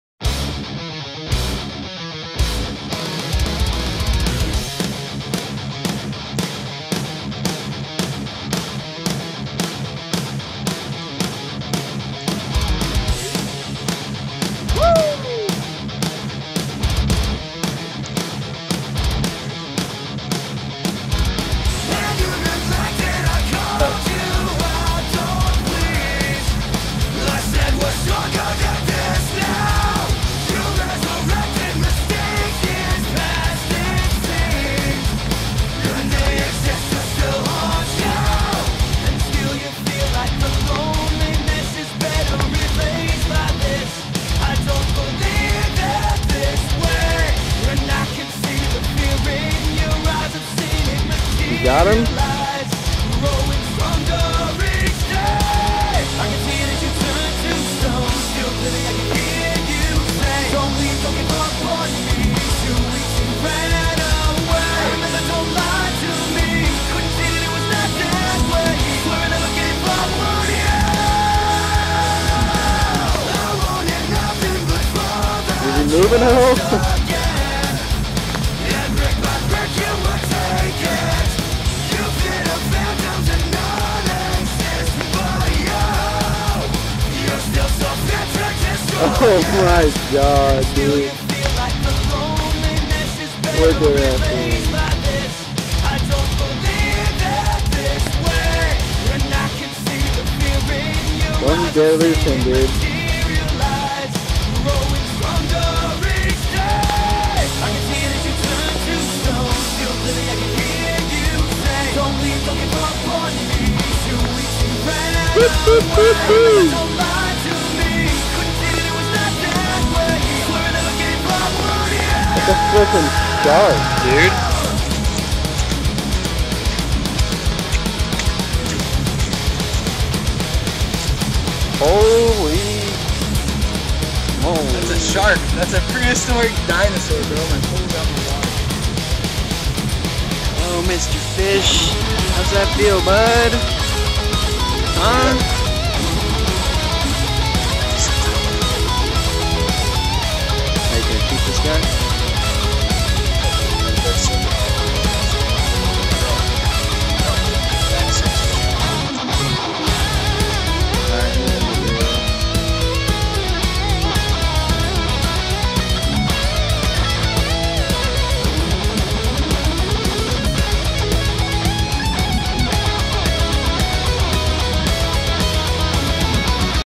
Fat Washington Sturgeon!!!
35 pound sturgeon, at secret spot, it put up a fight for about 45 minutes!
Gear: 6ft 6 berkley cherrywood hvy action rod, abu garcia cardinal reel, 10 pound spider wire.
Caught it on a crappie rig with nightcrawler.

At the end of the video he rapped himself up in a tree so i went in for him (: TIGHT LINES!!!